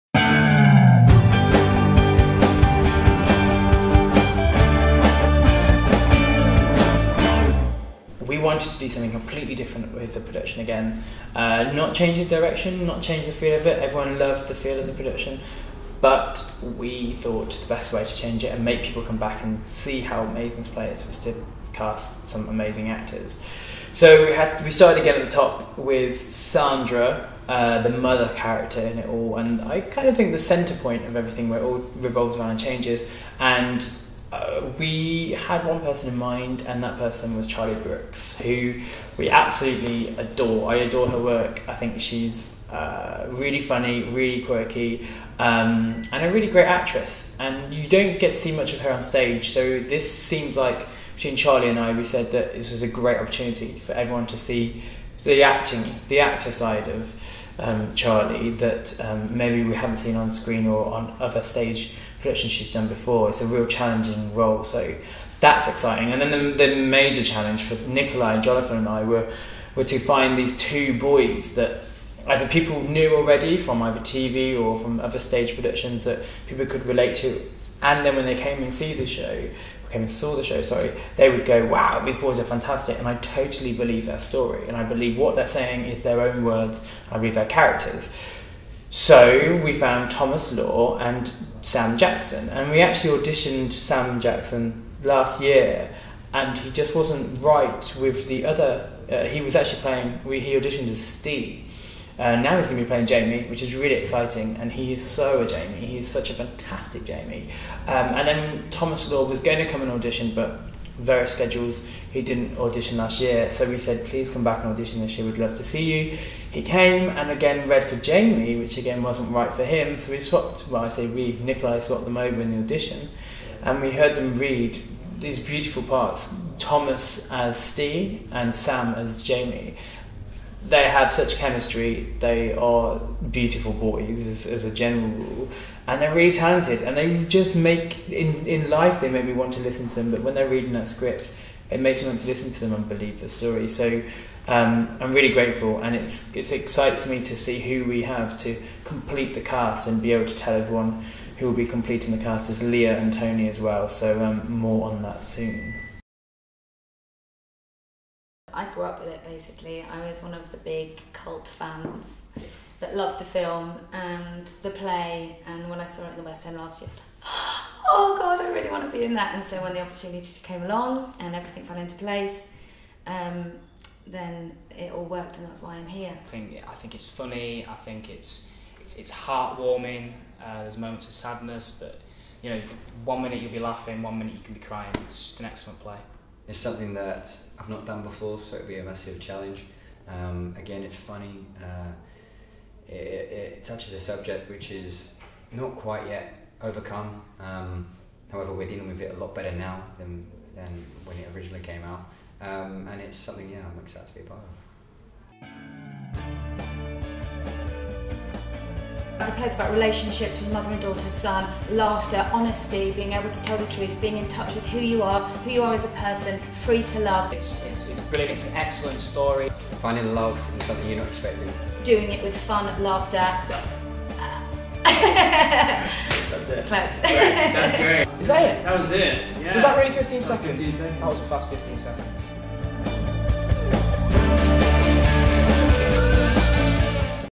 Beautiful Thing 2015 UK Tour - VLOG #1 - CAST ANNOUNCEMENT BEHIND THE SCENES
BRAND NEW CAST FOR MULTI AWARD-WINNING PRODUCTION:

Prepare to laugh and cry in equal measure as Nikolai Foster’s celebrated anniversary production of Jonathan Harvey’s classic 'Beautiful Thing' returns to the stage, with a brand new cast, touring the UK from 23 March 2015.

Multi-award-winning actress Charlie Brooks (EastEnders' Janine Butcher) stars in this glorious urban love story, of teenager Jamie’s relationship with classmate and neighbour, Ste. Together the two boys find comedy, warmth and the music of Mama Cass through their loud-mouthed next door neighbour, Leah. Featuring: Thomas Law (EastEnders) as Ste & Sam Jackson (Skins) as Jamie - this critically acclaimed award-winning play captures the bitter-sweet experience of being a teenager and falling in love.

HISTORY: Beautiful Thing was first produced in 1993 in the West End, the hit show became so popular a film was made in 1996 by Film4, and in 2013 the story celebrated its 20th anniversary - now that same production returns to the stage with a brand new cast.